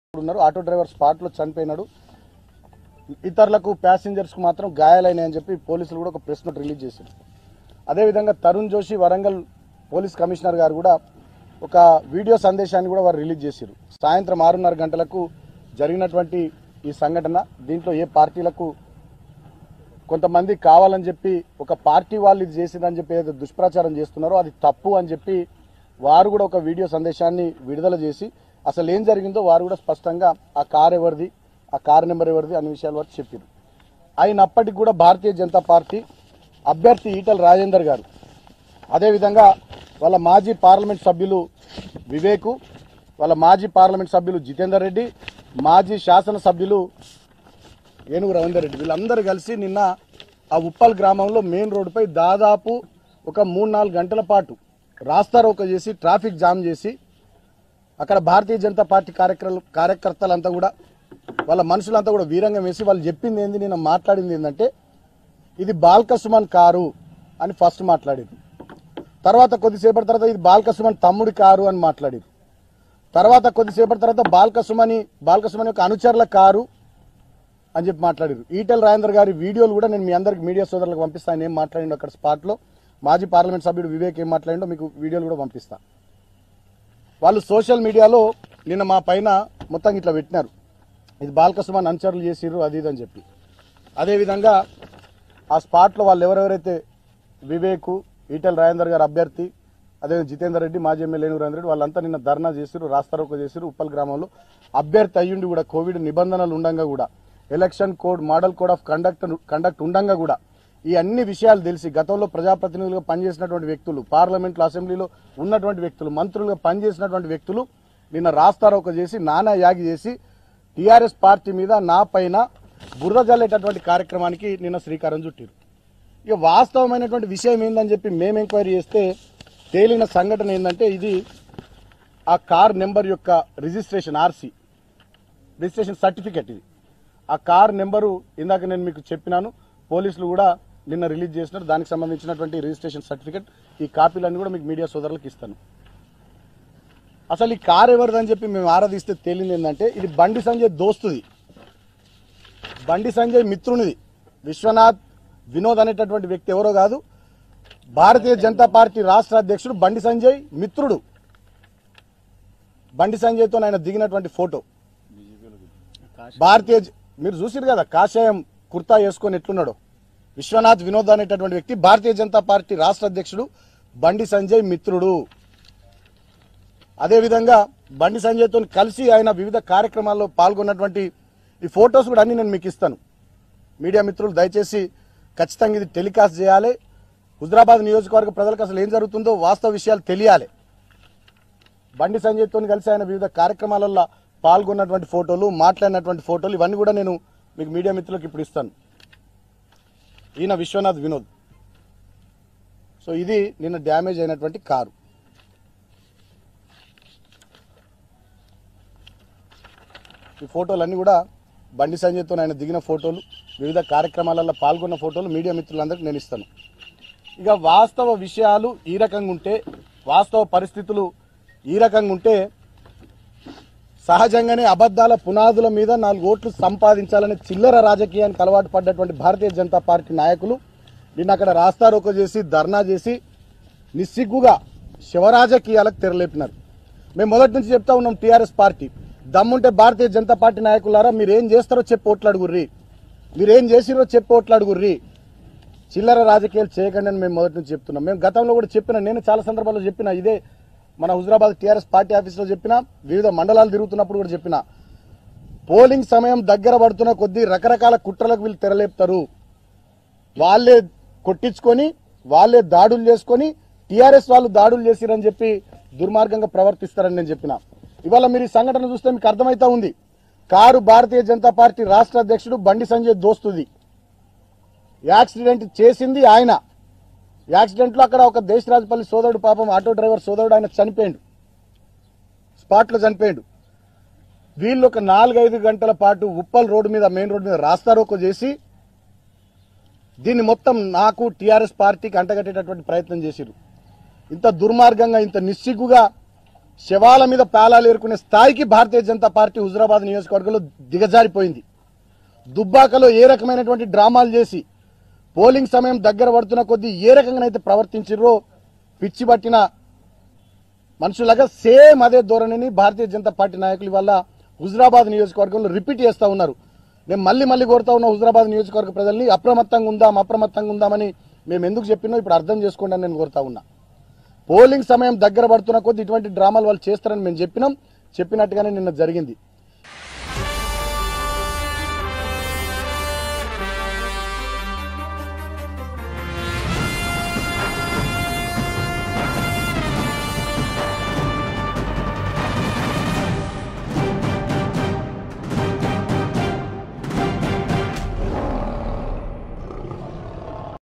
MLA Balka Suman Mass Warning to Bandi Sanjay and Etela Rajender | Huzurabad By Election | TRS
Leo Politics

Leo Health

Leo Entertainment

Leo Devotional